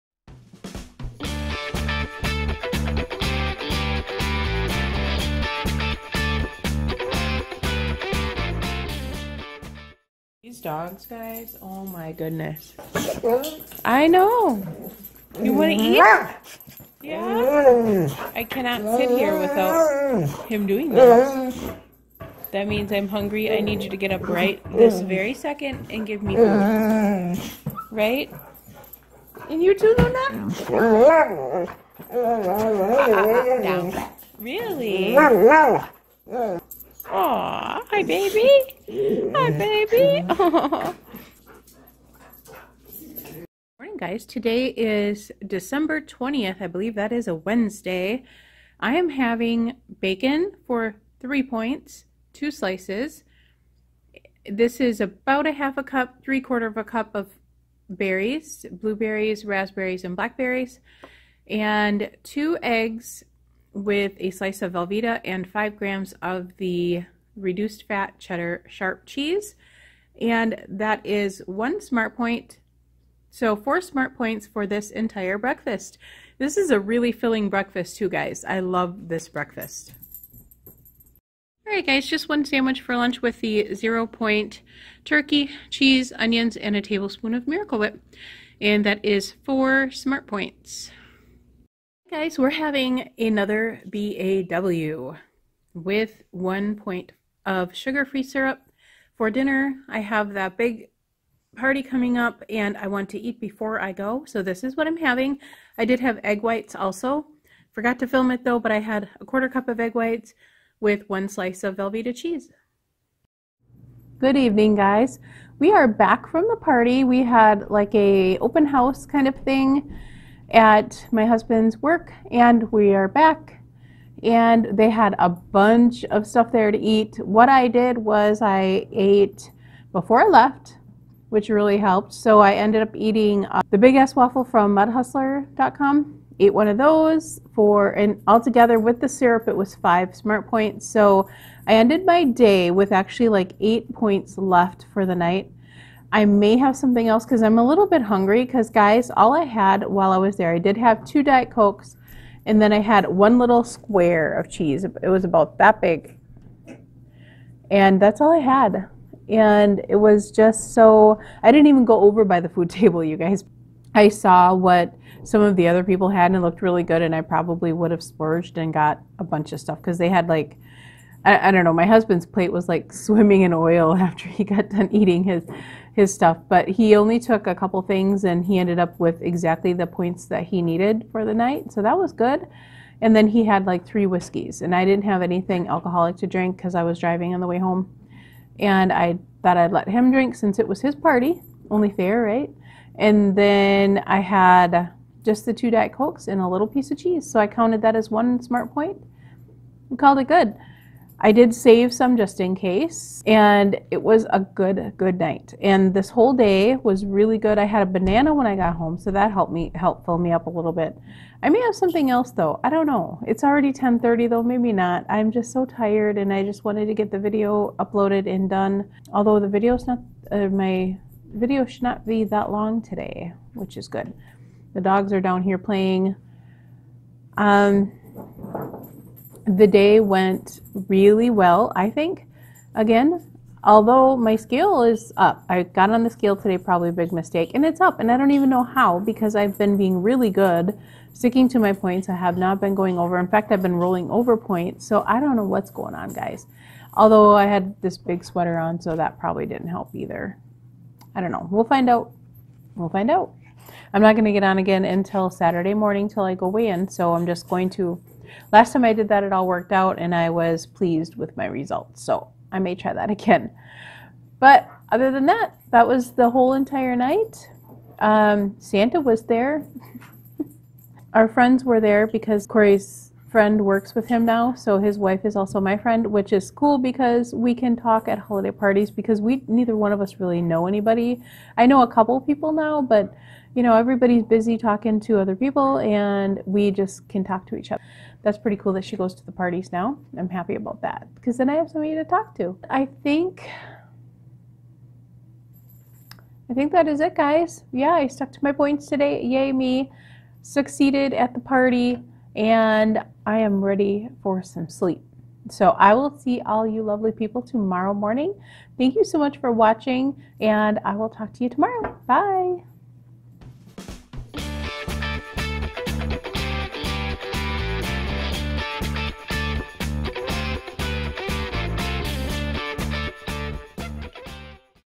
How did the work Party go? What I ate for the day on Dec 20 2017
SO...tonight we had a work Party to attend and it was an open house.  There were SO many good food items there...did I stay on track? Or did I WAY overdo it?  Check it out and see how I did tonight!